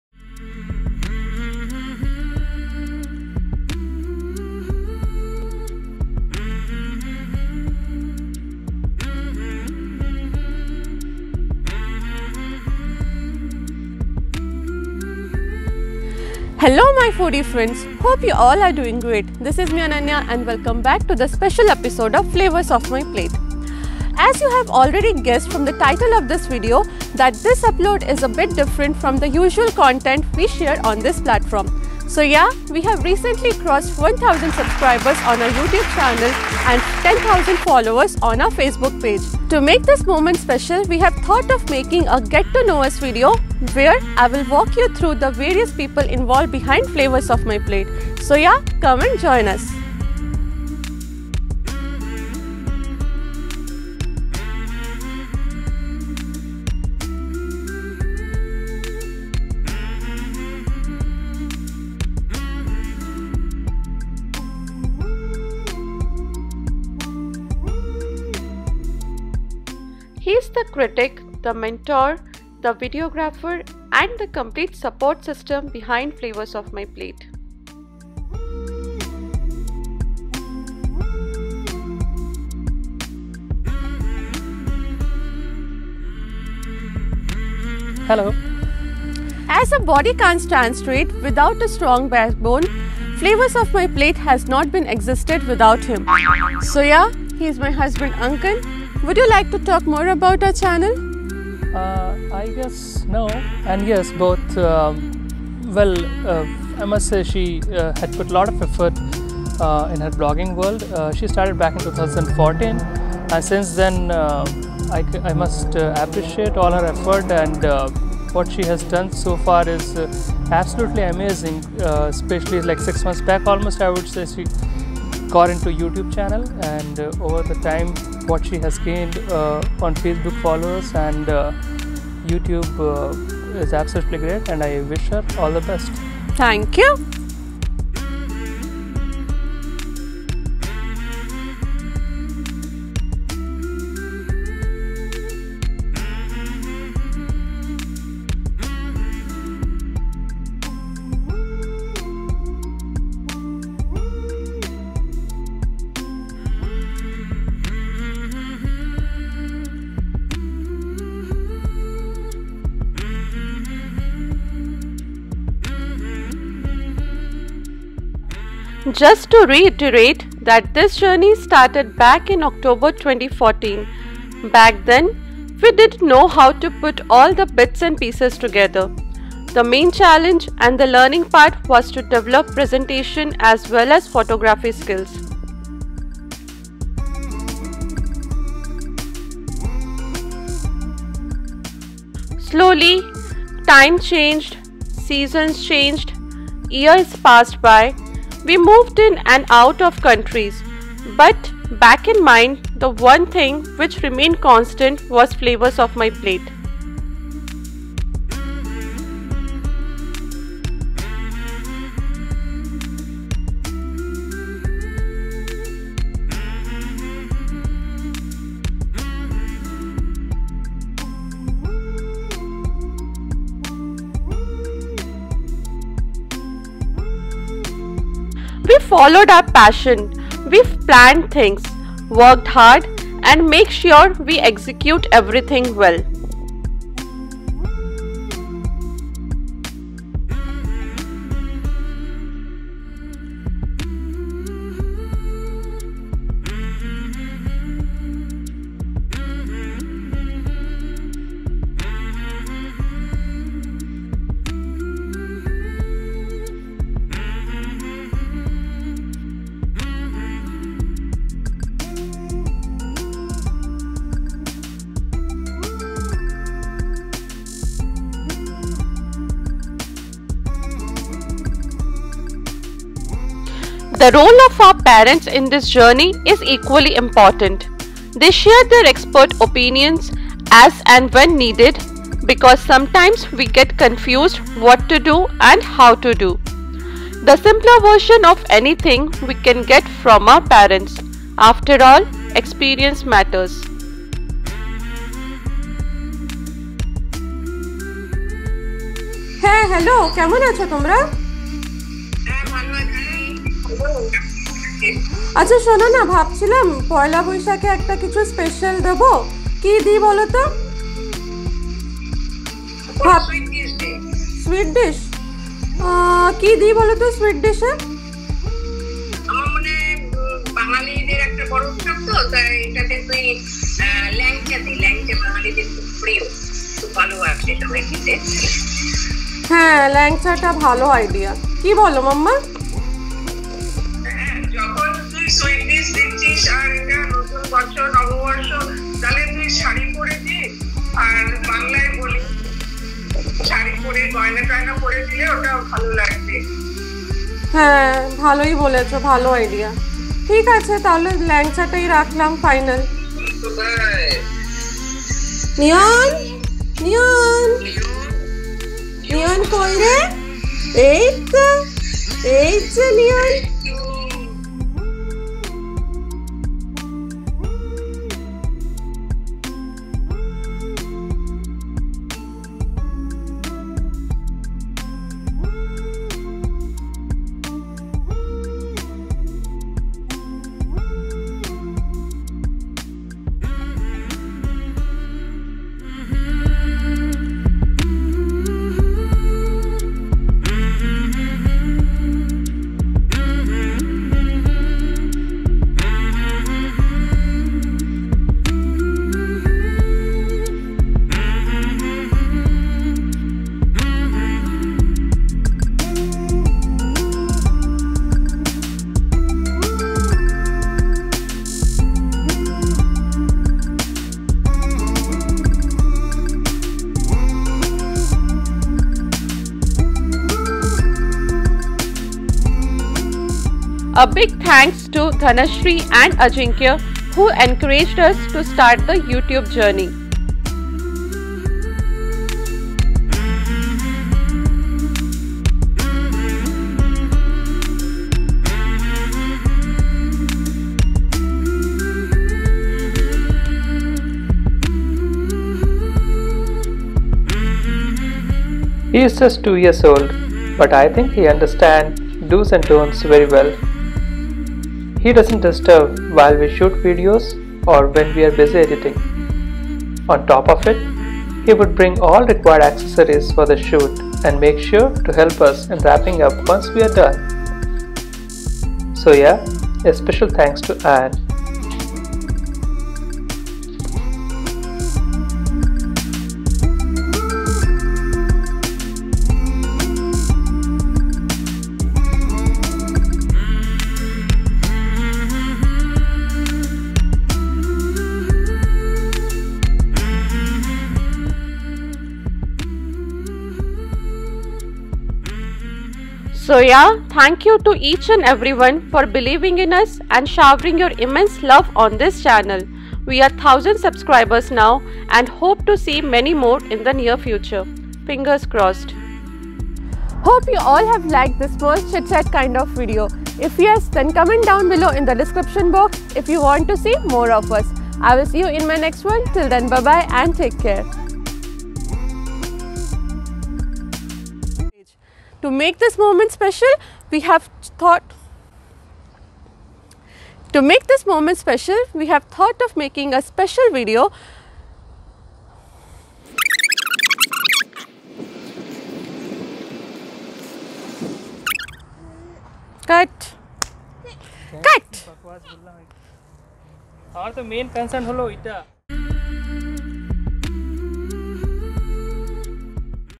1000 YOUTUBE Subscribers & 10,000 FACEBOOK Followers Special | Get To Know Us | Thank You Everyone
We hit the milestone of 1000 Subscribers and 10,000 Facebook Followers on the same day and i.e. April 9th 2019.

We started this journey back in October 2014 when we use to do blog post. We were doing well on that platform but slowly and gradually it took a back seat in our life. Until last year when we thought of starting a YouTube Channel.

So yeah, Thank You to each and everyone for believing in us and showering your immense love on this channel. We’re 1000 subscribers now and hope to see many more in the near future. Fingers Crossed.